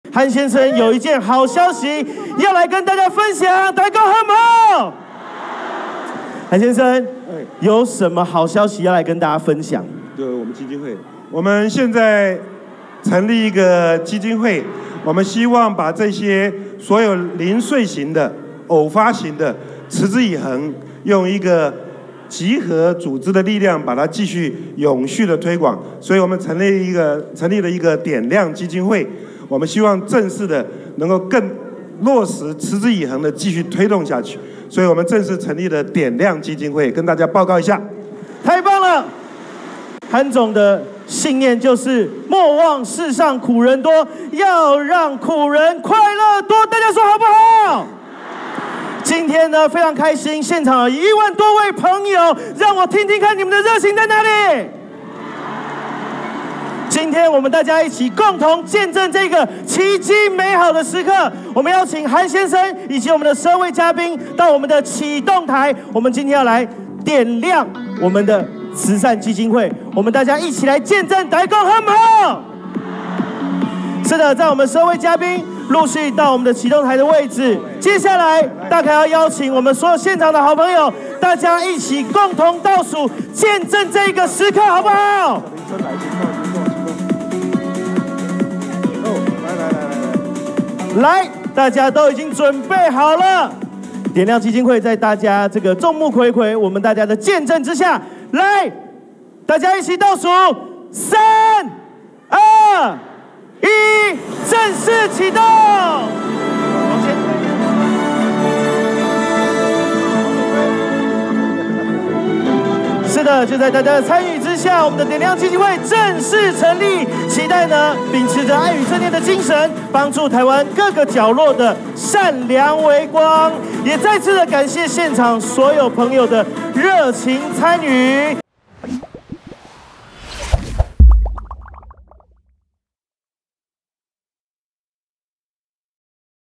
韓國瑜宣布成立「典亮基金會」 盼永續推廣良善力量｜中時新聞網
高雄市前市長韓國瑜今(2)日在大安森林公園舉辦《韓先生來敲門》新書見面會，大批韓粉漏夜排隊擠爆現場。最後，韓國瑜也在公佈了一個好消息，他成立了「典亮基金會」，自己接任董事長一職，希望能夠把這些零碎型的、偶發型的良善，用一個集合組織的力量將它推廣，讓美麗的台灣、善良的台灣、正能量的台灣，能夠揚善全球。

【相關新聞】
上萬韓粉擠爆見面會 韓國瑜：台灣的希望在於草根人民